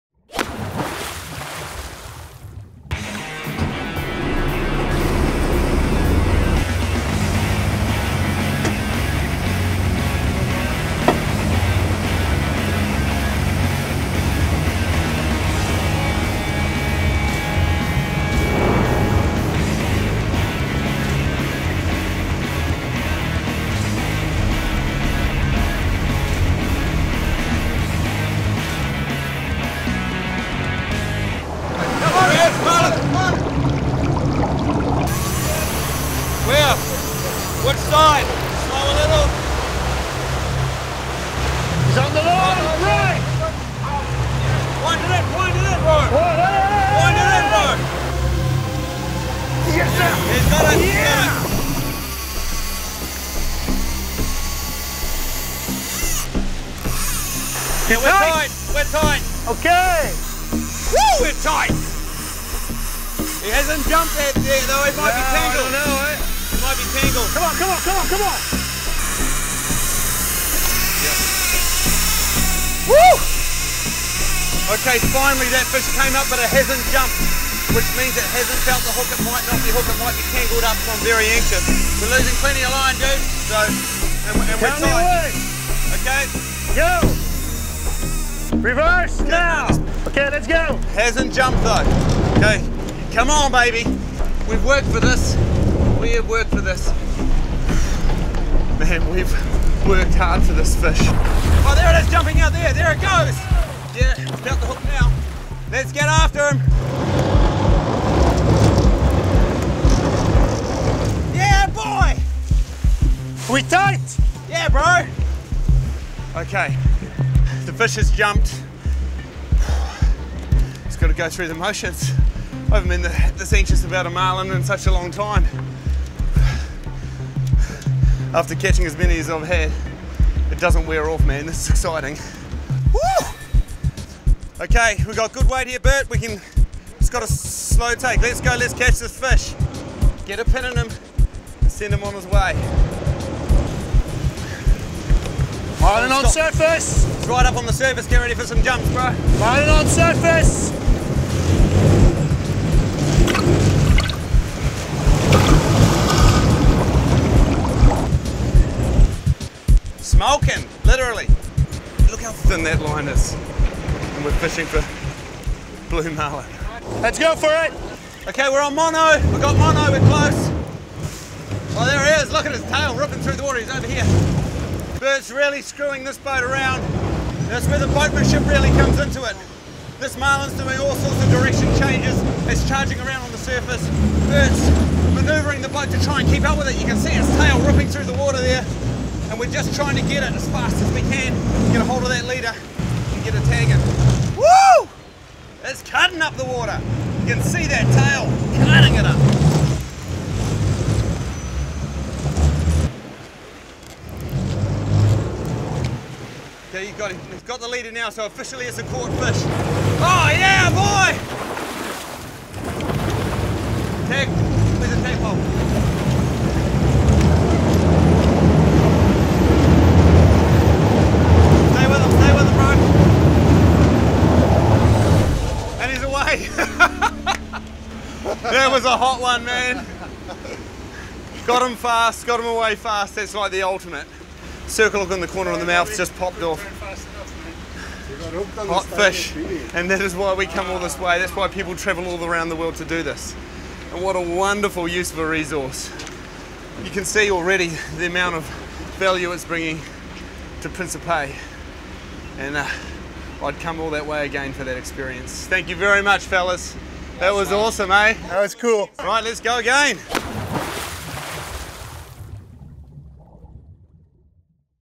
BAIT AND SWITCH BLUE MARLIN - PRINCIPE
BONUS FOOTAGE - Bait and Switch on a Blue Marlin while fishing Principe - this footage that didn't make the big screen

Matt and the crew switch baiting marlin in the waters off West Africa.